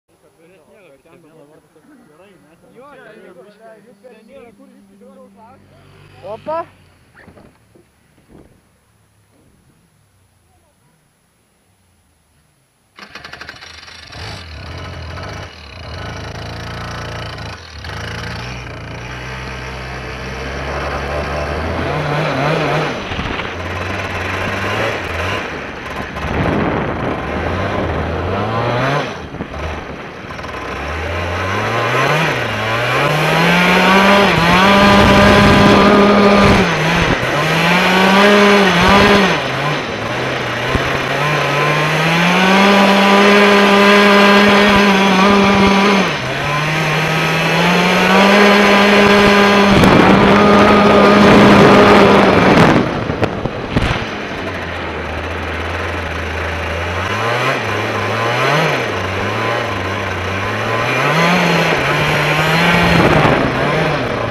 Ice karting in Kaunas
Short clip of ice action in Kaunas, during the winter in Lithuania.